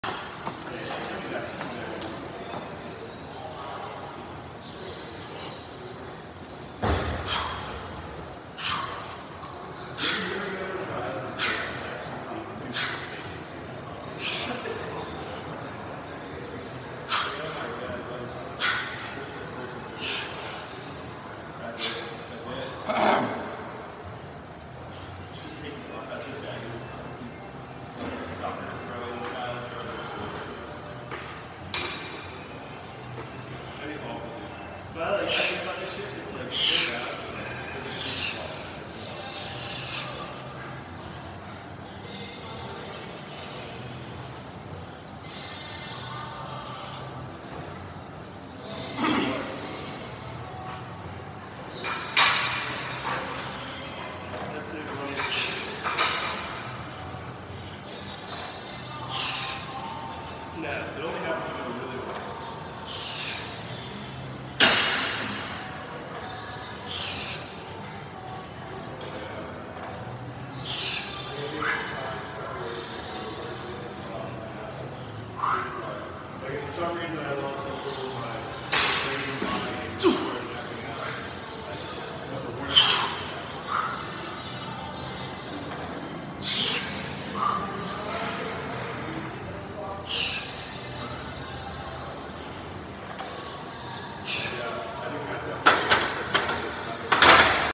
225x25 Platz Style High Bar Squat
First time trying, keeping balance is difficult. Gonna take another few sessions before I do anything cool